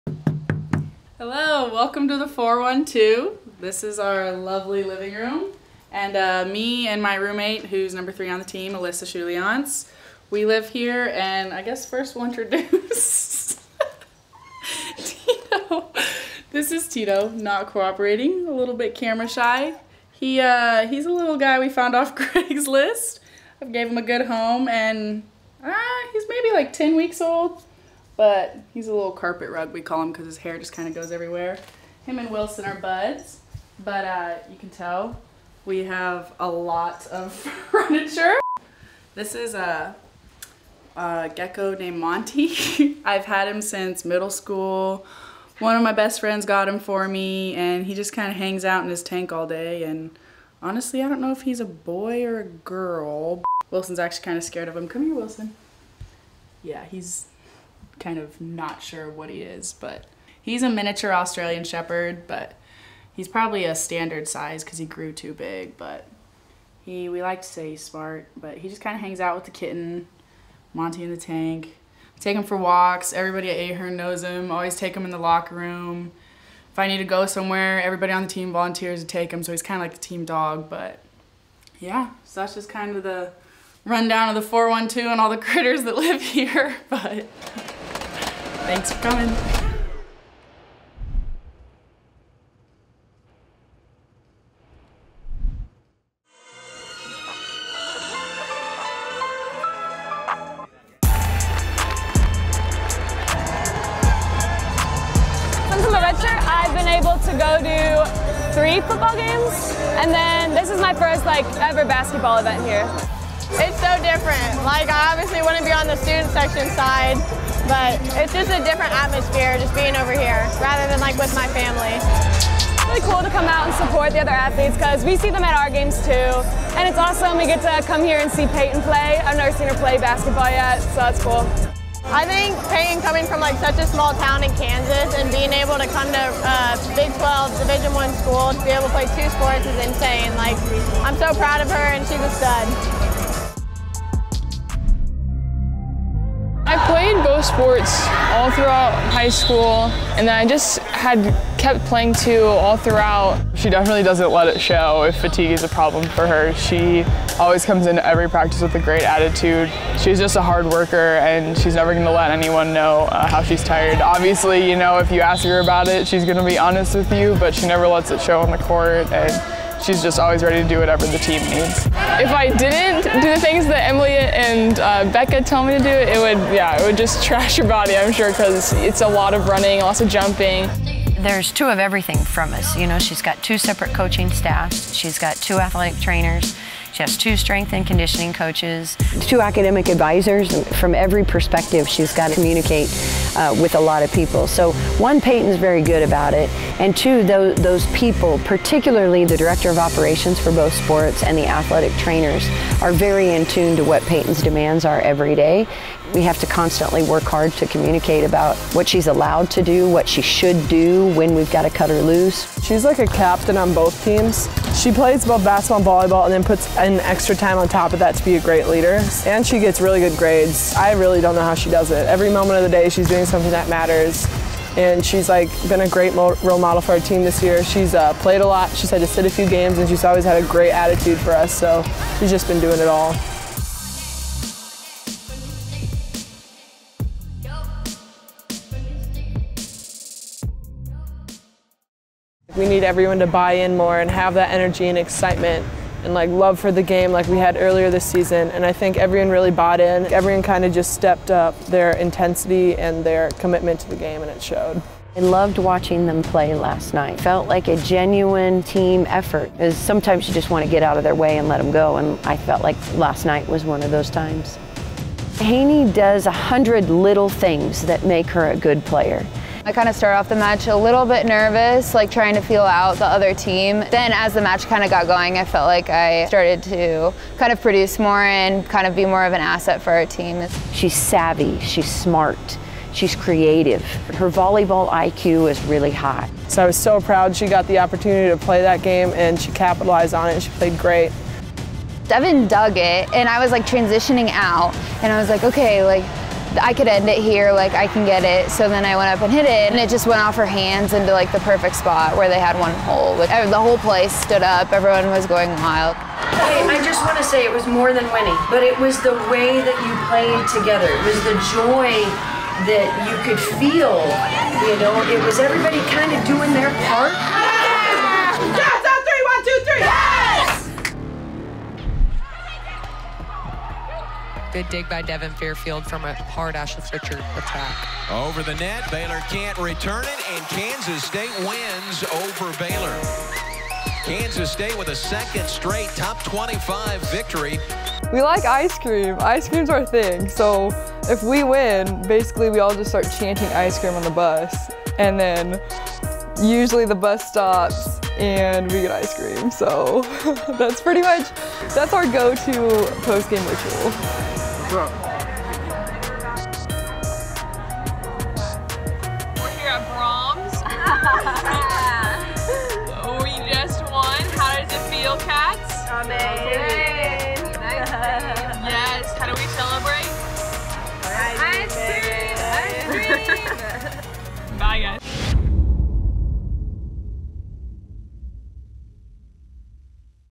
K-State Volleyball | Volleycats - Episode Six
In this episode of Volleycats, we meet up with the team at "Madness In Manhattan" as they cheer on teammate Peyton Williams, we get to know Macy Flower's pets, and recap wins over KU and Baylor.